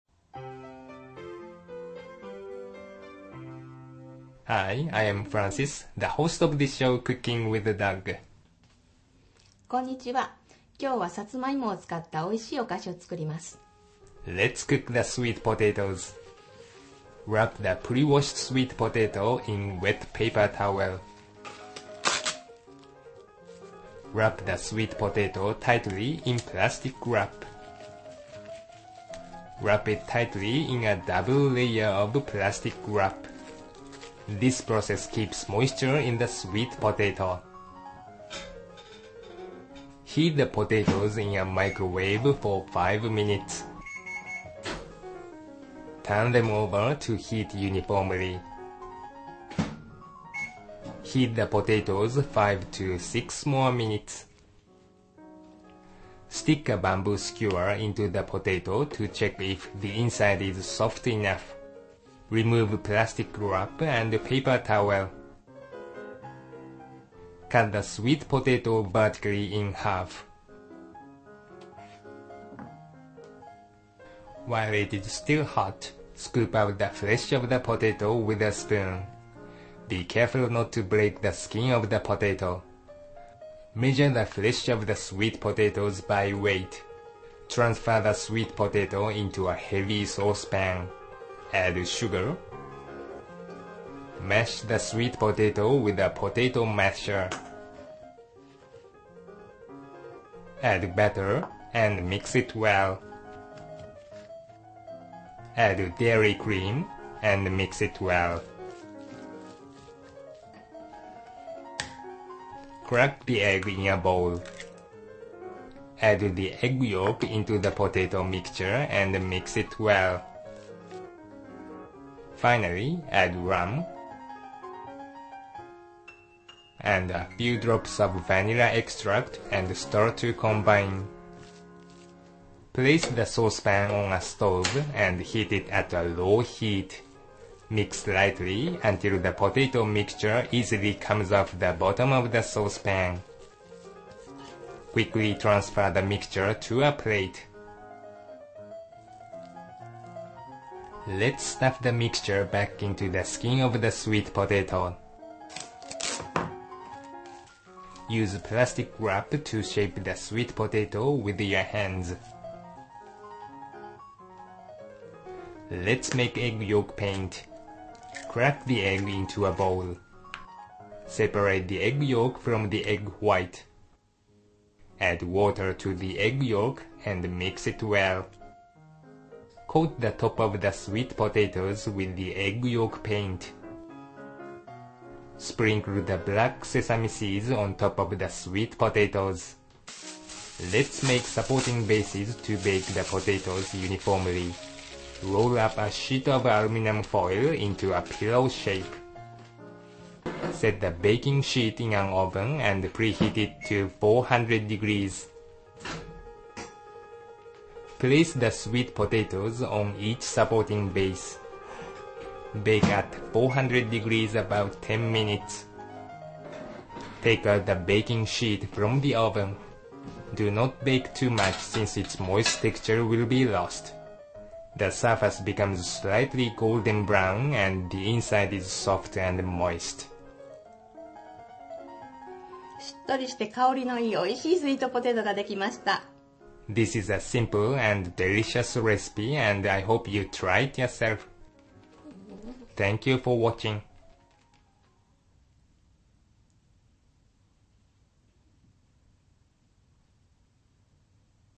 Sweet Potato Cake Recipe (Moist Aromatic Autumn Dessert Made From Sweet Potatoes) | Cooking with Dog
We are making a scrumptious autumn dessert made from sweet potatoes. This dessert is simply called Sweet Potato in Japan, and it is moist, aromatic and very delicious.

(4 pieces) Cook Time: 40 minutes

2 Sweet Potatoes (gross weight 500g/1.10 lbs, net weight 300g/0.66 lbs)
50g Sugar (1.76 oz)
20g Butter (0.71 oz)
60ml Dairy Cream (0.254 u.s. cup)
1 Egg Yolk
1/2 tbsp Rum
A Few Drops of Vanilla Extract

1 Egg Yolk for Gloss Finish
1/2 tsp Water

* 1 tablespoon (tbsp) = 15 ml, 1 teaspoon (tsp) = 5 ml

** Note: Reduce the microwave time to 3 minutes on each side of the potatoes for a full sized American microwave. The microwave we use (500 watt) also works as a conventional oven. Cook sweet potatoes in a microwave for the first time. Then bake them at 400 degrees Fahrenheit (200 degrees Celsius) in a conventional oven.

Search your favorite Cooking with Dog recipes!

Help translate our recipes and get our original Mug, Chopsticks and T-shirt!